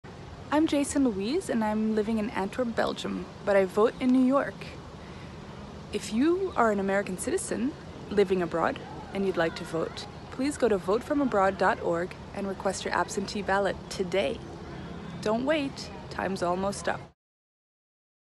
Vote in New York from Abroad - Jason-Louise Graham
Jason-Louise lives in Antwerp and votes in New York - join her by requesting your US absentee ballot